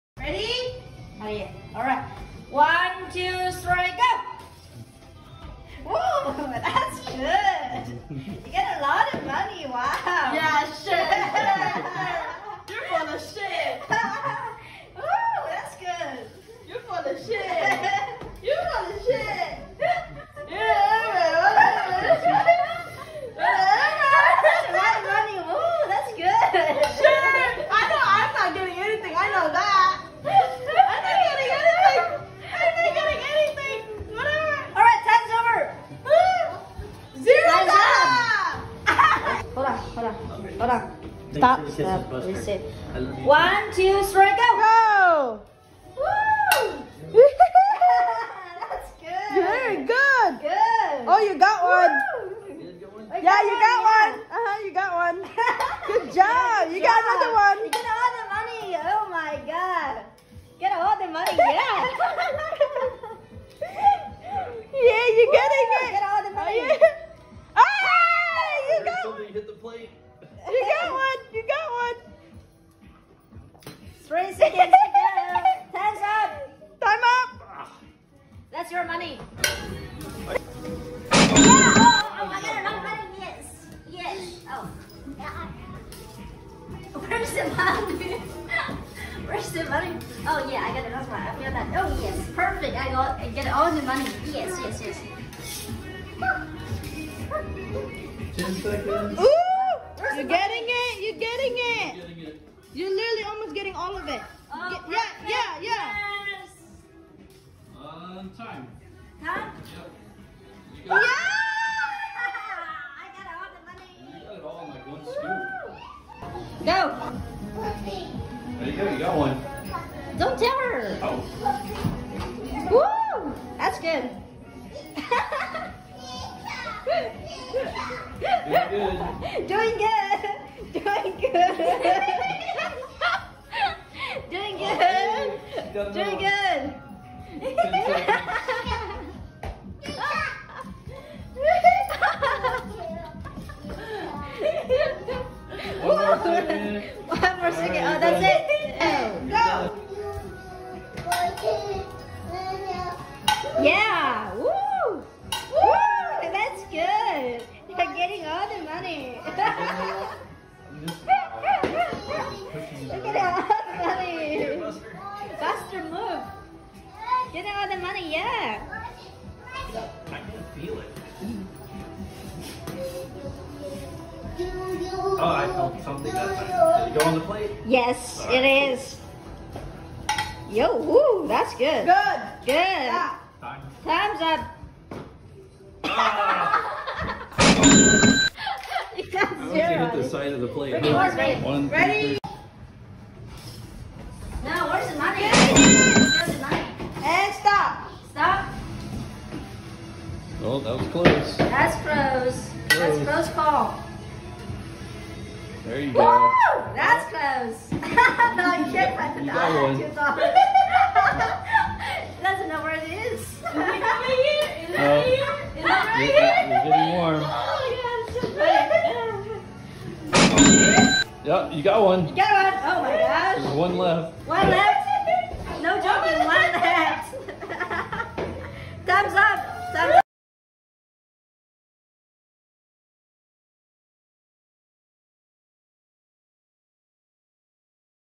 Playing the Instagram money game with my friends
I saw a video on Instagram where friends were competing for money by scooping it up with a spatula and dropping it on a plate, 30 seconds per turn.  We each chipped in $10... I won the most money $21 and Clint was the last place with just $1.  Buster kept messing with Clint so it wasn't his fault.